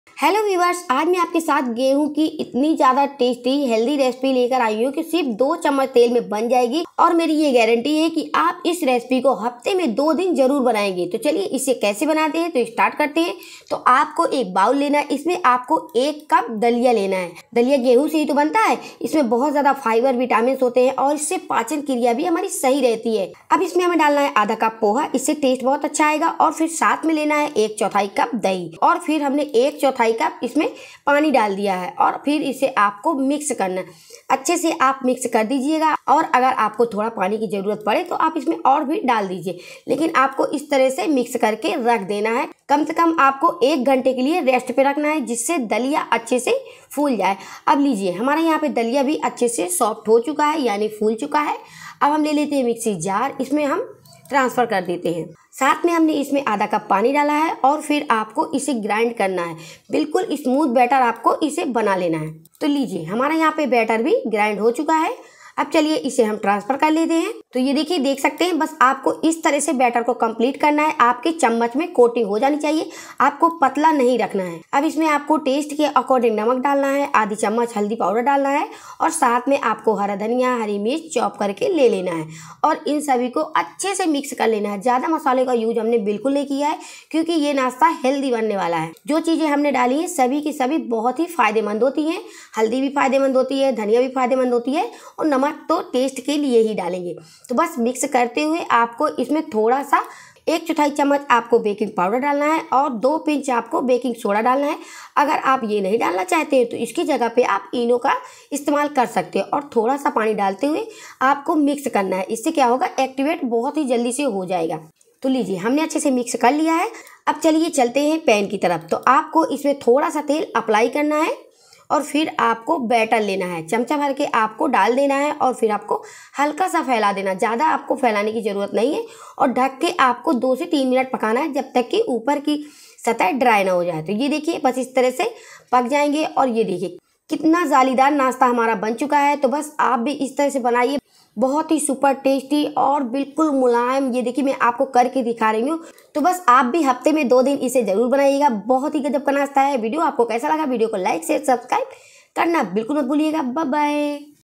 दलिया से बनाये दुनिया का सबसे हेल्दी एंड टेस्टी नाश्ता हफ्ते में 4 दिन बनाएंगेTasty nasta|Breakfast
ना अप्पम ना चीला पराठा ये तो है दलिया का सुपर सॉफ़्ट टेस्टी एंड हेल्दी नाश्ता | Tasty nasta